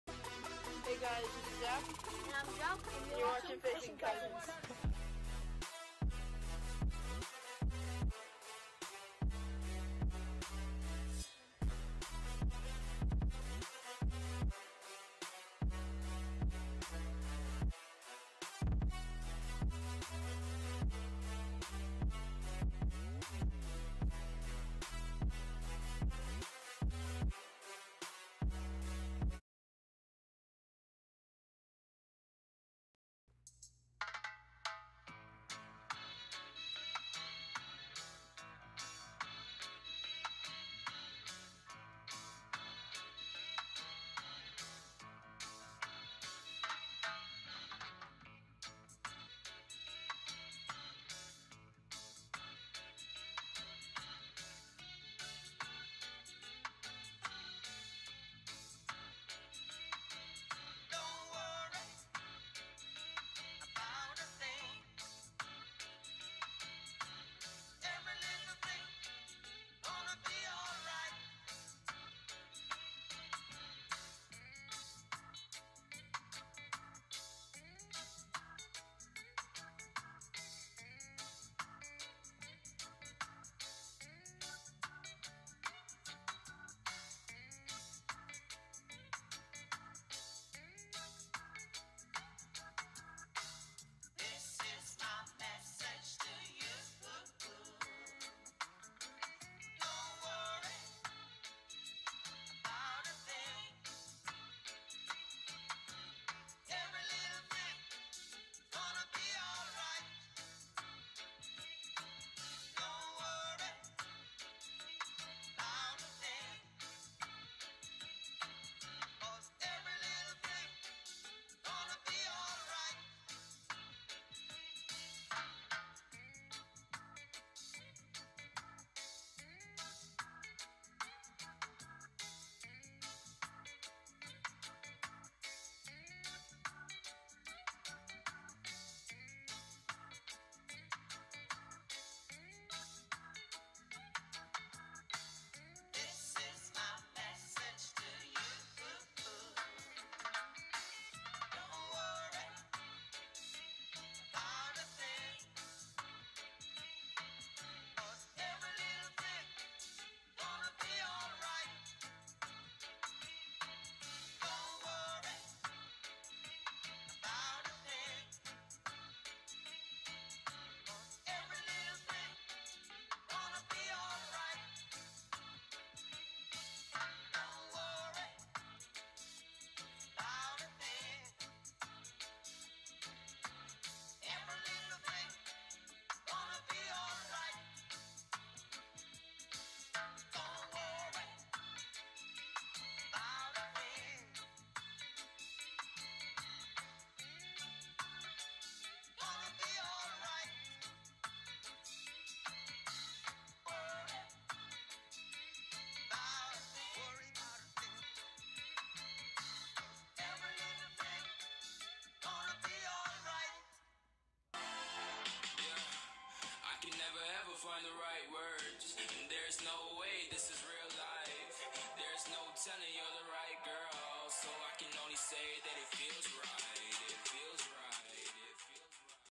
SNORKELING THE REEF IN LAUDERDALE BY THE SEA!! (TARPON, SHARKS, TURTLES)
In this video we go snorkeling right off the beach in Lauderdale By The Sea, Florida. Filmed by Joe’s Dad and brother (Peter and Nick).